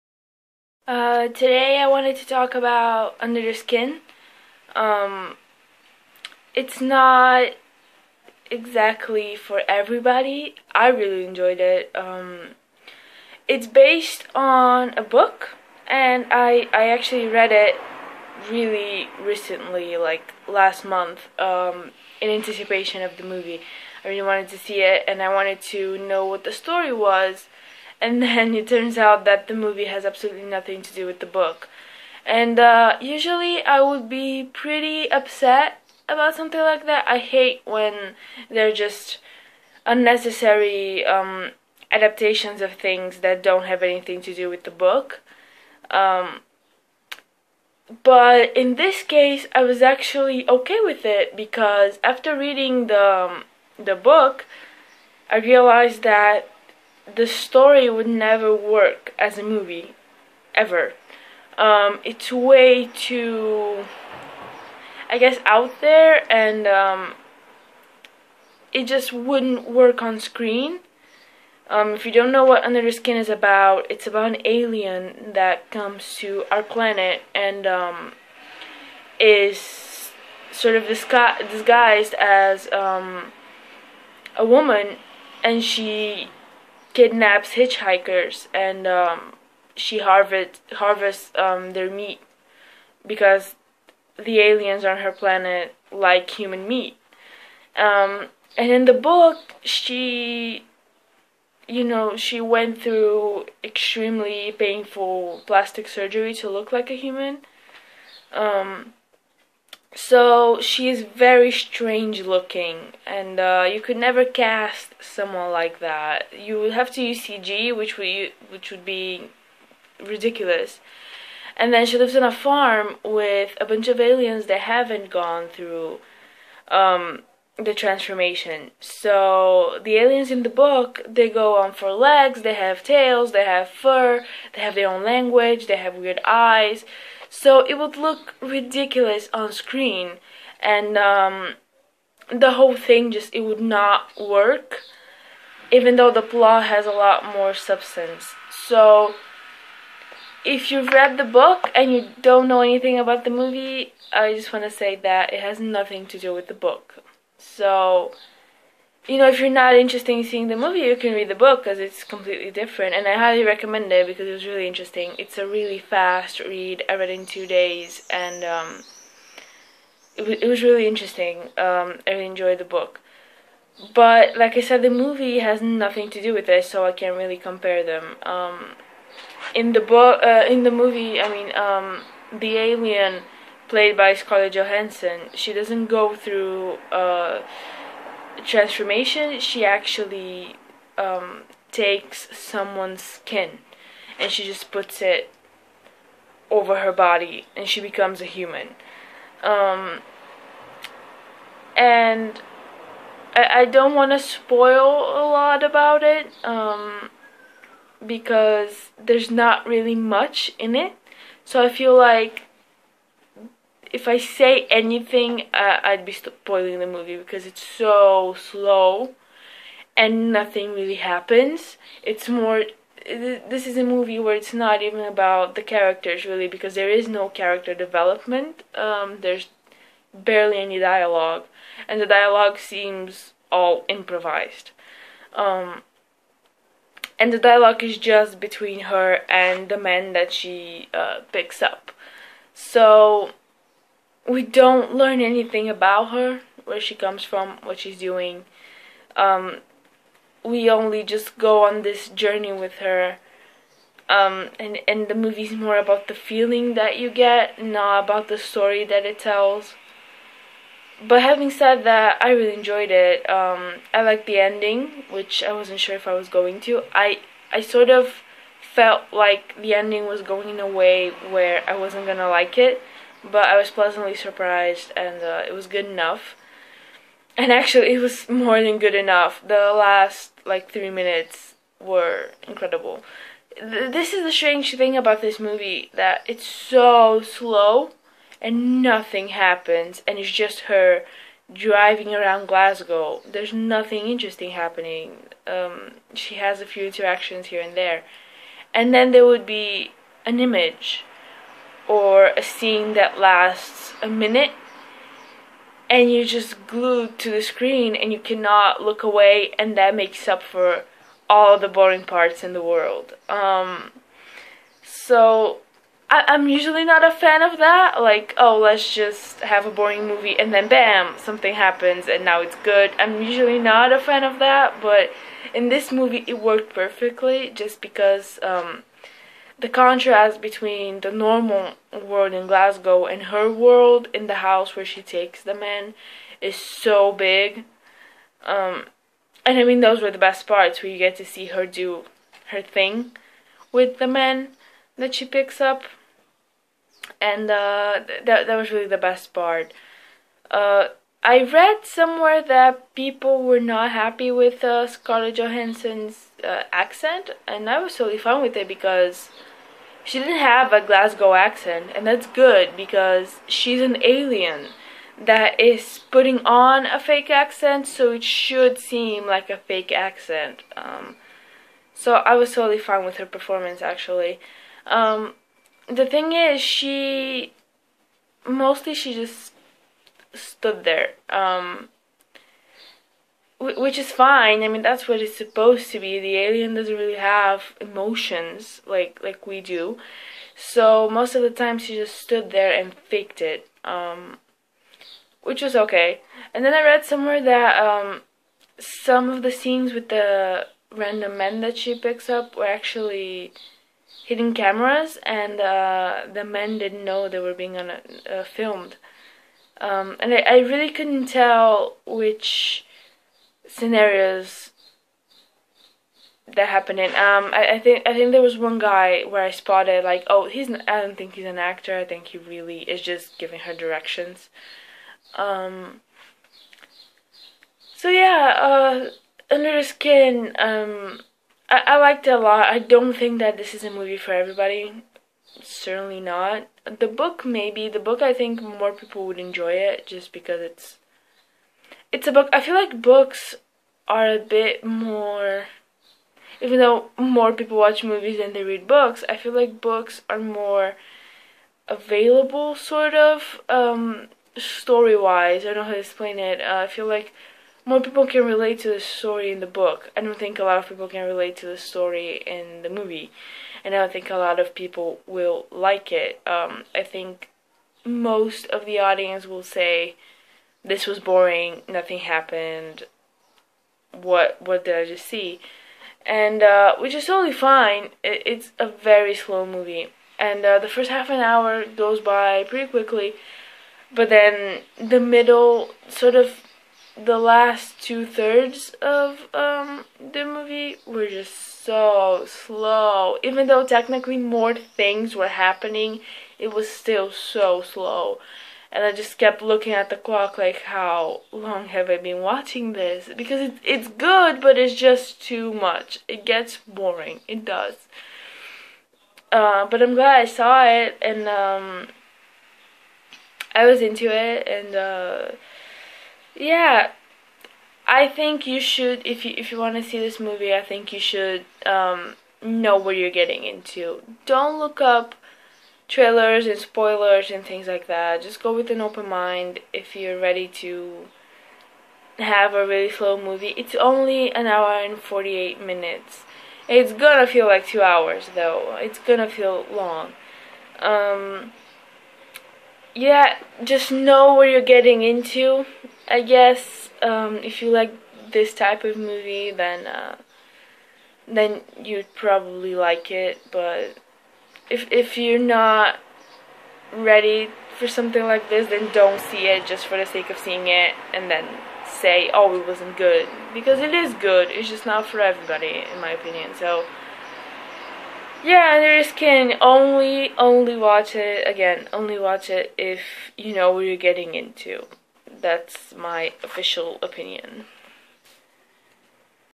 Under The Skin Review
A mysterious seductress preys upon the population of Scotland.